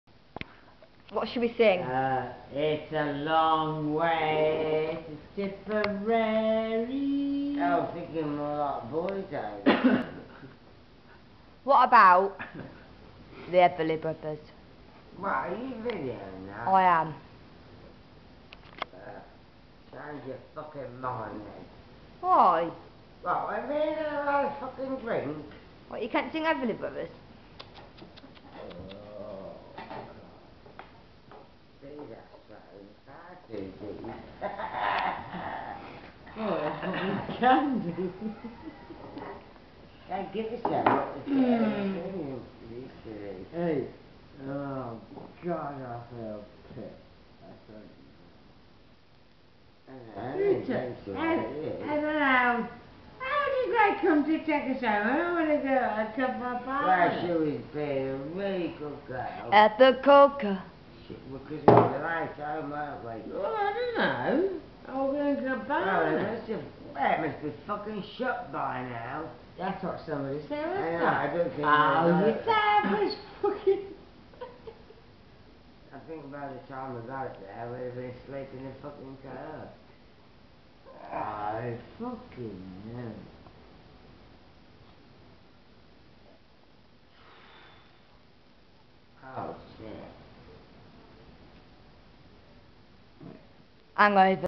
Drink and laughter.AVI
Two friends after a night out drinking